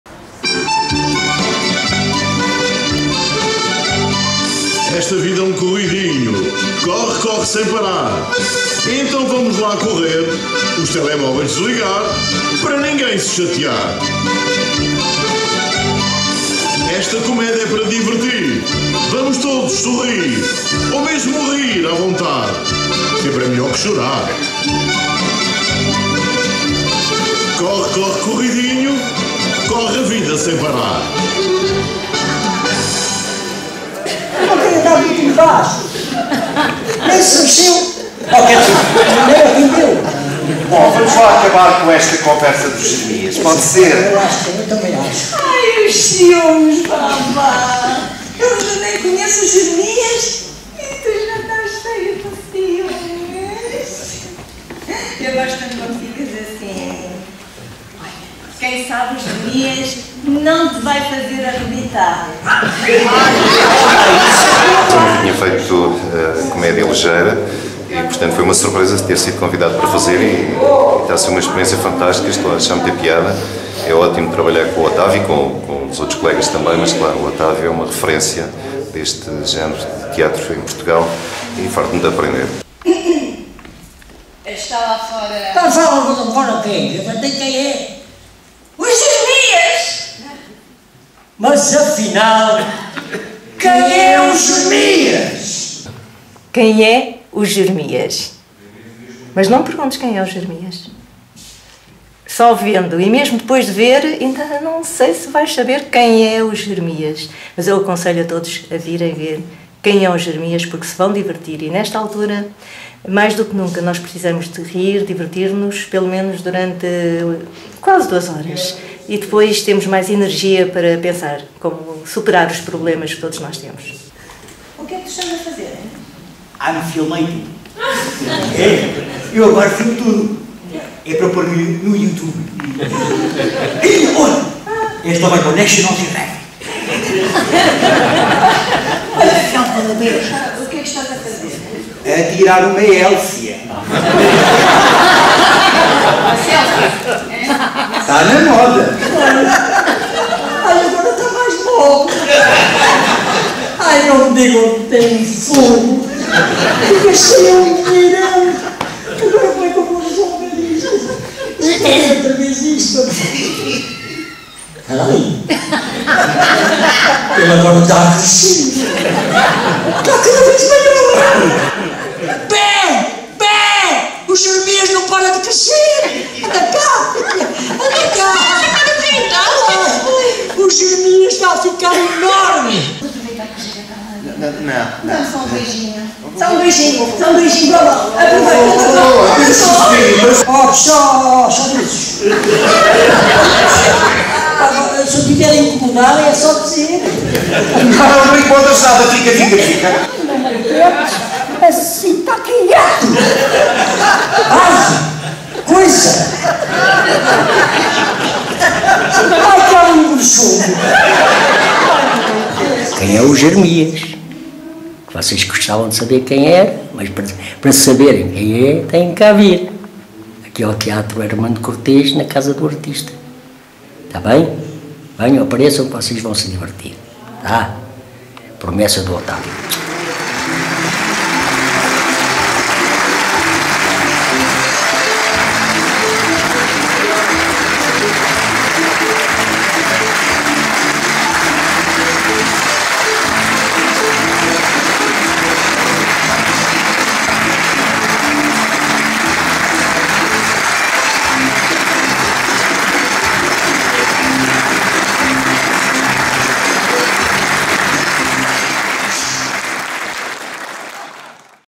"QUEM É O JEREMIAS ?" com OCTÁVIO MATOS - 5.12.2014
REPORTAGEM WEBTVP
REALIZADA POR NUNES FORTE
NA ESTREIA EM LISBOA
DE “QUEM É O JEREMIAS?”

Espetáculo protagonizado por OCTÁVIO MATOS com ÁLVARO FARIA E ISABEL DAMATTA que além de atriz também encenou e é igualmente a autora com o pseudónimo TOMÉ LISBOA; participam ainda JOAQUIM GUERREIRO E HELDER CARLOS.

Esta divertida comédia preparada para digressões não só no país, mas também nas comunidades portuguesas espalhadas pelo mundo, está em cena em Lisboa durante Dezembro de 2014 no TEATRO ARMANDO CORTEZ ./ CASA DO ARTISTA.